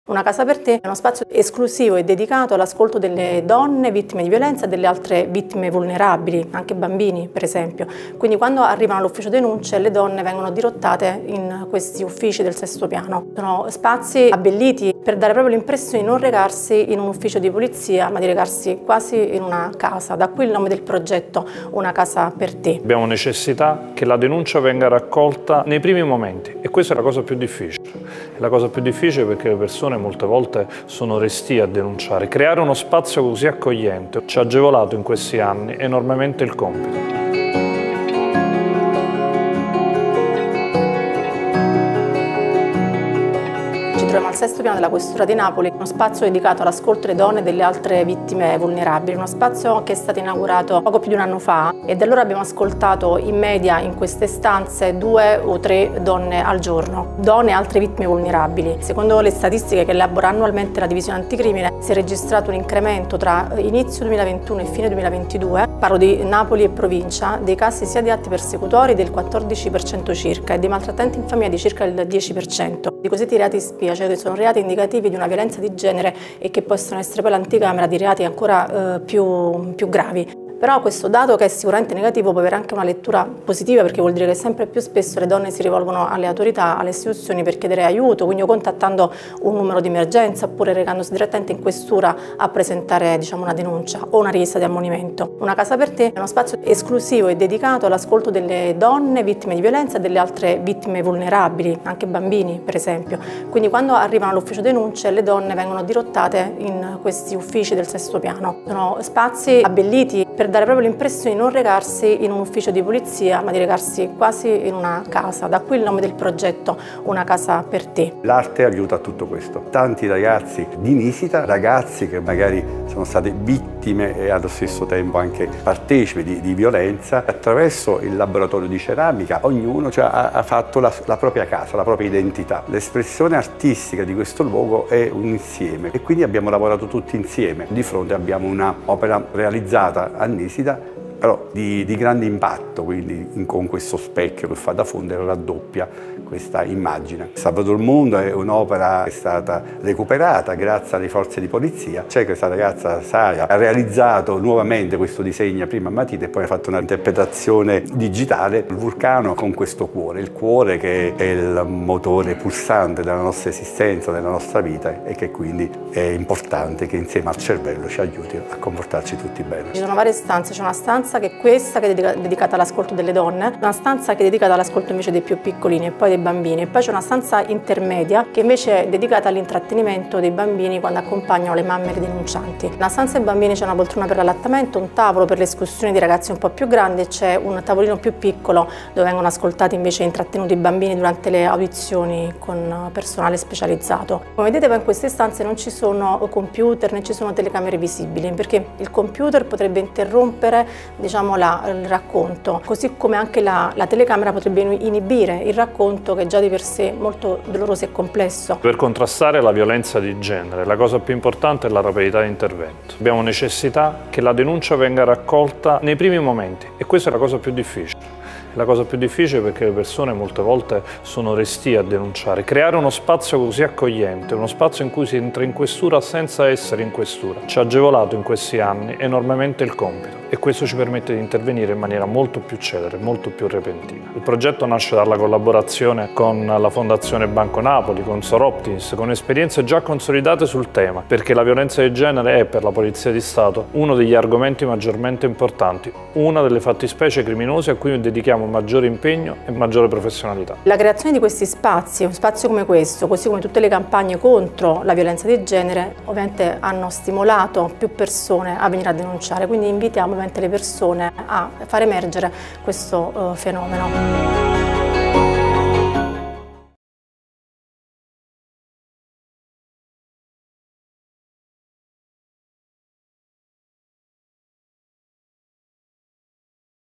Napoli, una "casa" per le donne vittime di violenza
Al sesto piano della questura di Napoli, c'è uno spazio dedicato alle donne vittime di violenza e ai soggetti vulnerabili. Tra le opere di Lello Esposito, stanze accoglienti e adibite all'ascolto di donne e bambini. Angela Castaldo, Vice Questore della Polizia di Stato, il dirigente della Squadra Mobile di Napoli, Alfredo Fabbrocini, e Lello Esposito ci spiegano come è stato realizzato il progetto e quali sono le sue finalità.